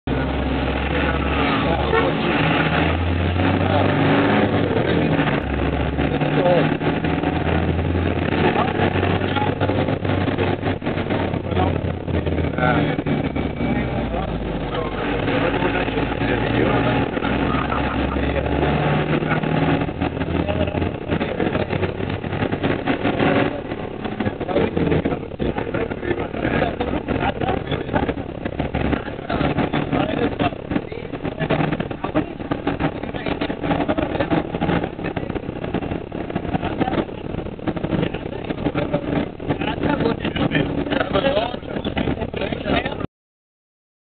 Ethiopian drive 7
At the West side of the Blue Nile Gorge, now turning East again, in the villige a coptic church is being constructed, or maybe renovated.